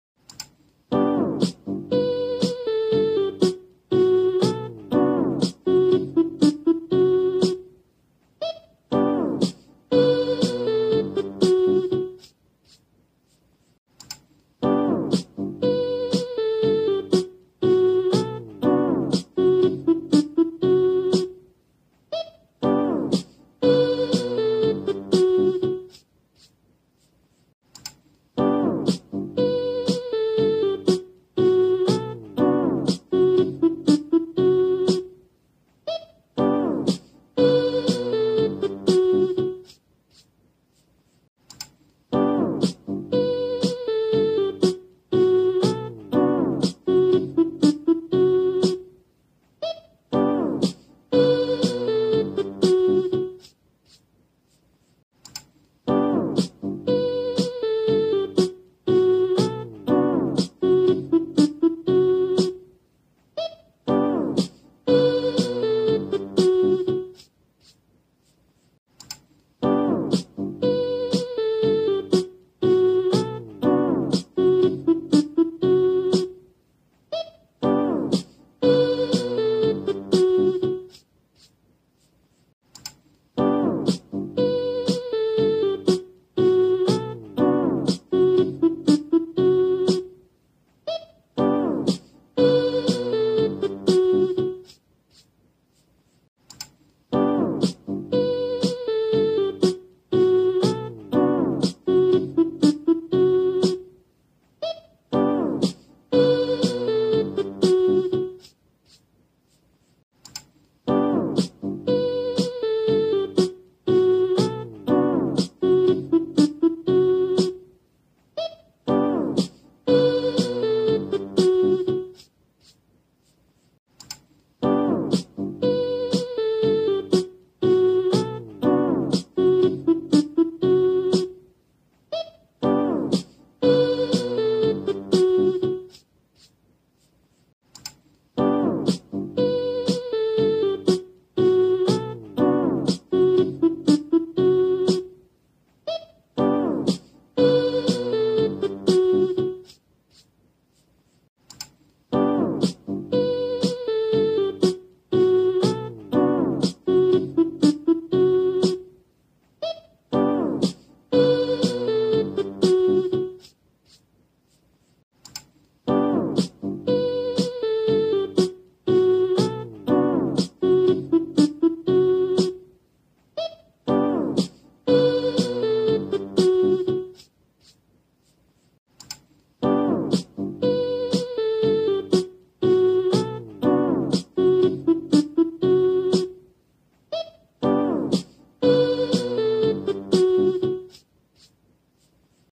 페럿춤 3분 영상 (Ferret Dance 3minute)
댓글에서 많은분들이 긴영상을 원하셔서
짧은영상 붙여넣기로
3분으로 만들었습니다